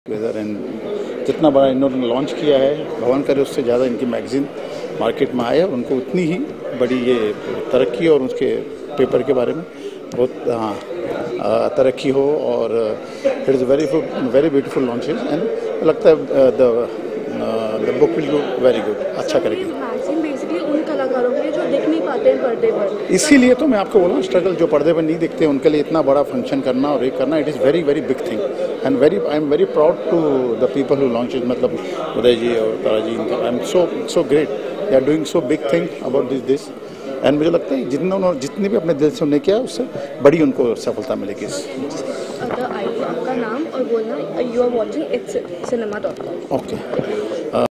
Tinu Verma Interview at launch of "It's Cinema.com"
Tinu Verma gives best wishes to "It's Cinema" Magazine and portal.